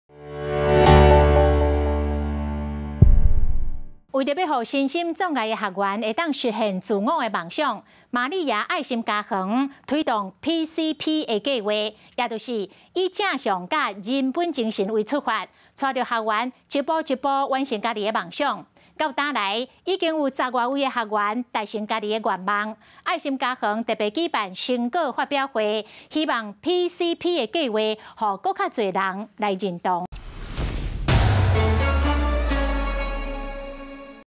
愛心家園PCP計畫 助學員按計畫完成夢想｜華視台語新聞 2022.11.22
為了讓身心障礙學員，有實踐自我夢想的機會，瑪利亞愛心家園推動PCP計畫，以正向和人本精神為出發，帶著學員一步步完成自己的夢想，如今10多位學員已完成自我夢想，這天，愛心家園特地舉辦成果發表會，希望PCP計畫能被更多人認同。